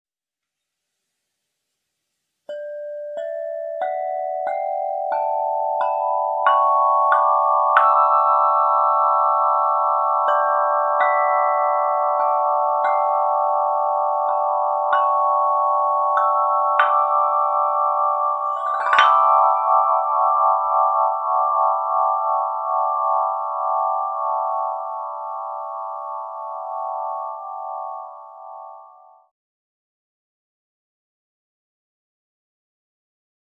Divya Waterfall. Sound Healing, Meditation, Medicine Ceremony. Chimes bells Chakraphone
Divya is a unique musical instrument (a metallophone for sound therapy) designed and created by us at Divya Svara. It's perfect for those who practice yoga, meditation, sound therapy, medicine ceremonies, and more.

• *Waterfall - D E F# G A B C# D E (D major)*
• Waterfall scale gives sensations: fluidity, plasticity, and flexibility. The ability to transform into any form: solid cold ice, flowing blue water, warm stream, or a refreshing waterfall.

Divya is presented in _eight scales,_ each with its own mood and state, offering a unique "sound aroma composition."
This instrument brings happiness, an elevated state of joy, lightness, and unforgettable sunny emotions, making you feel like you've entered a wonderful fairy tale or a magical dream.

_Divya is the Temple of Sound that is always within reach._